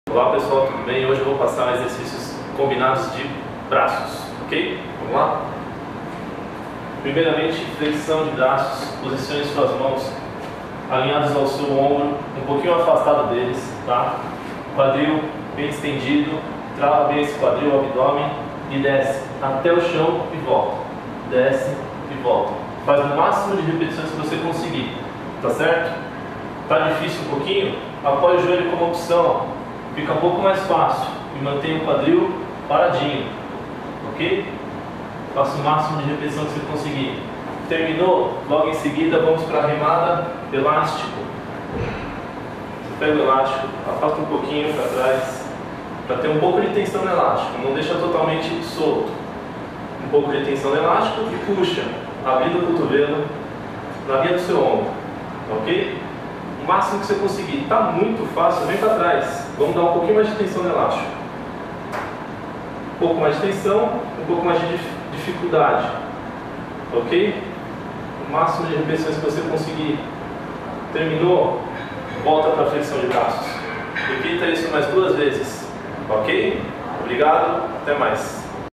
Iniciando com Pique Total.
Serie Combinada: Flexão de cotovelos e remada elástico. Sem intervalo.
Gravado na academia Activa -- Av. Henrique Shaumann, 717 -- Pinheiros
Produção: Deborah Worn